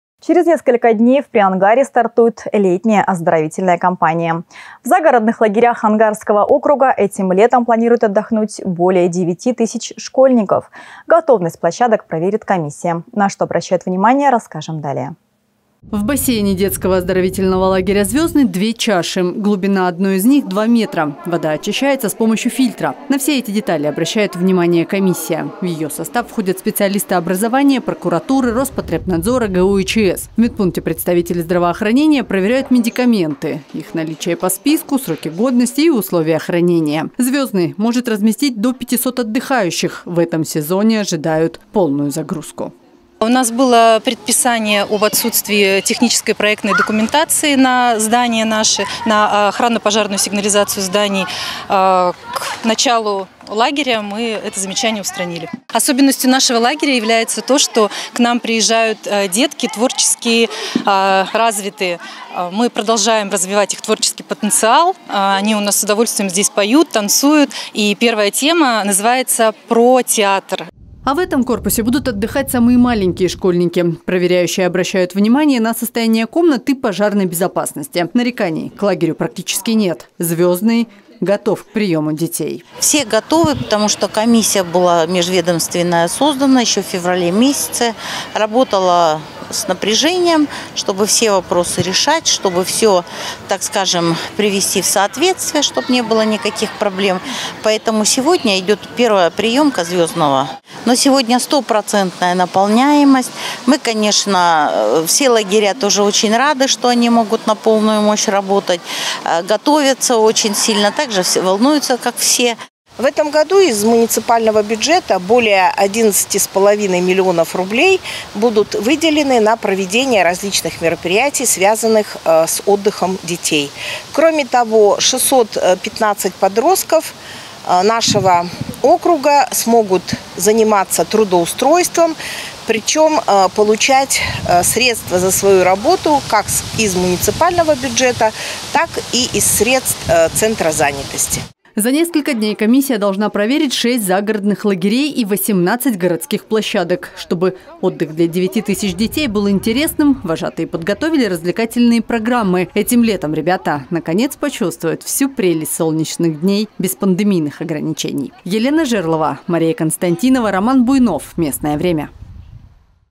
Все готовы?
Через несколько дней в Приангарье стартует летняя оздоровительная кампания. В загородных лагерях Ангарского округа этим летом планируют отдохнуть более 9 тысяч школьников. Готовность площадок проверят комиссия.